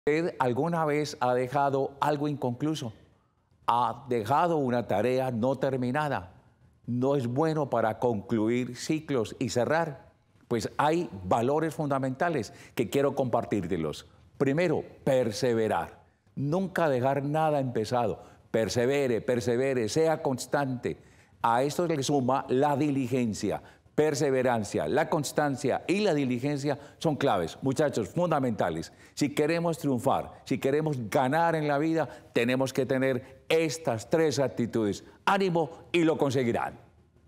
Perseverancia, Constancia Y Diligencia - Palabra De Dios
Cosmovision te invita a compartir con nosotros momentos religiosos y espirituales, acompañados de Padre Jaime Humberto Henao Franco,
Sacerdote Arquidiócesis de Medellín